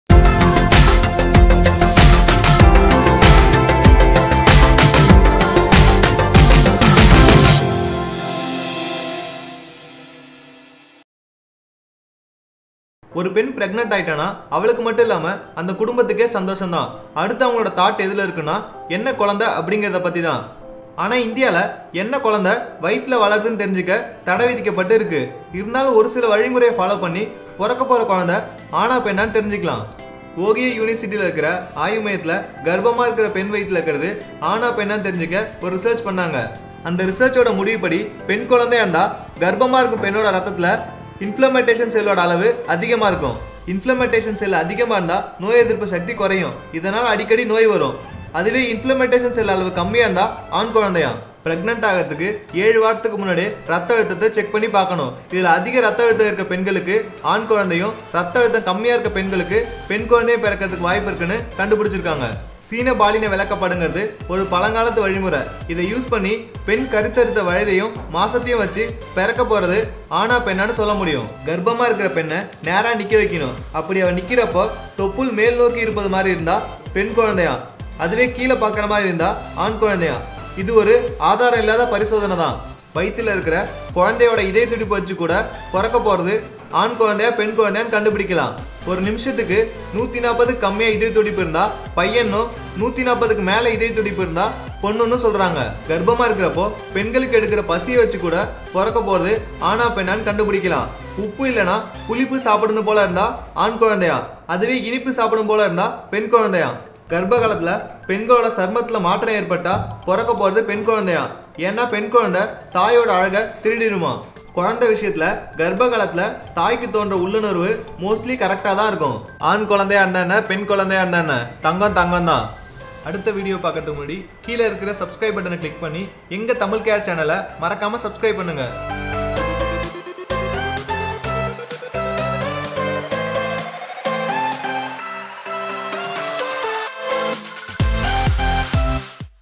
பிறக்கபோவது ஆணா பெண்ணா தெரிஞ்சிக்கறது எப்படி? | Tamil Health Tips | Home Remedies | Latest News
Steps to find Gender of baby in pregnancy

பிறக்கபோவது ஆணா பெண்ணா தெரிஞ்சிக்கறது எப்படி?

When the girl is getting pregnant whole family are very interested to know baby is boy or girl. But in India there are some restrictions to know it. There are some natural methods to find it. In this video we are going to see about it.

TamilCare is one stop destination for useful health and beauty tips in Tamil language.